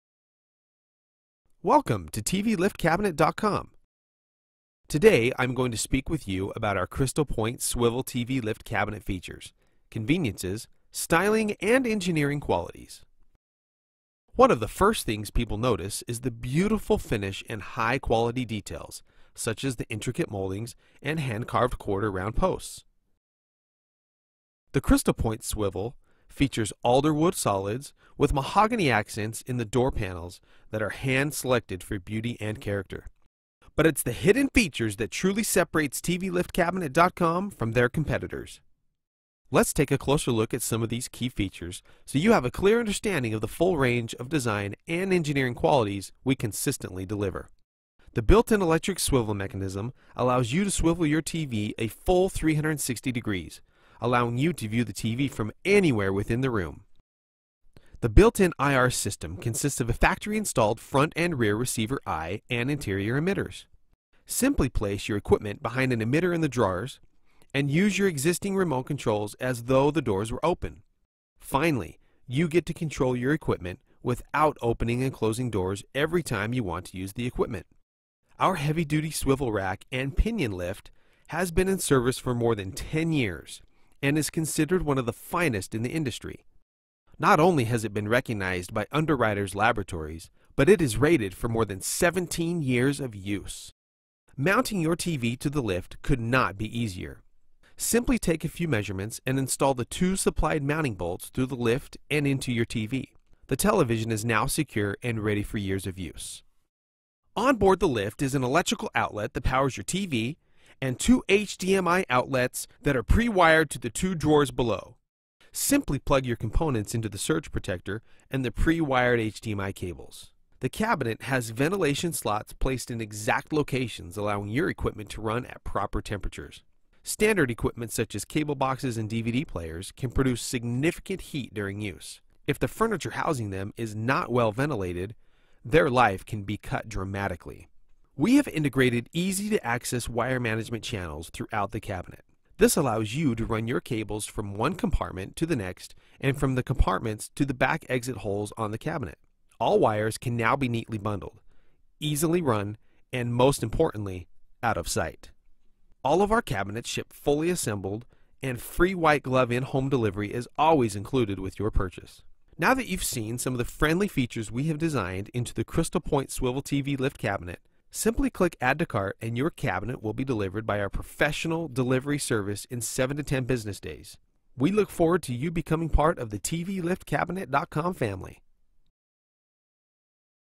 Crystal Pointe Brown 360 Swivel End of Bed TV Lift Cabinet
Finished on all four sides, there's no mistaking the Crystal Pointe 360 Swivel TV lift's classic design heritage with its intricately carved corner pillars, balanced proportions and finely crafted Mahogany veneer.

This TV lift cabinet is finished on five sides, and it's unique design allows the component drawers to be reversed so they open from the front or the back -- you choose! This enables the Crystal Pointe to be positioned against a wall, as a room divider, or at the foot of a bed, and still have full access to the screen and the component drawers - it's three products in one. Plus a 360 degree electric swivel! Crystal Pointe is grandly scaled and elegantly designed, inspired by antique European furniture.

The Crystal Pointe motorized TV lift features a warm brown finish made from select solid woods with mahogany veneer. An integrated heavy-duty, rack and pinion drive swivel TV lift mechanism silently lowers the television into the console.

Our motorized TV lift console units are equipped and preinstalled with a built-in Infrared Relay System consisting of the main unit, flush mounted infrared sensor, and four high output flashers.

The TV and all audio-visual equipment are completely controlled when out of sight and without ever opening the drawers!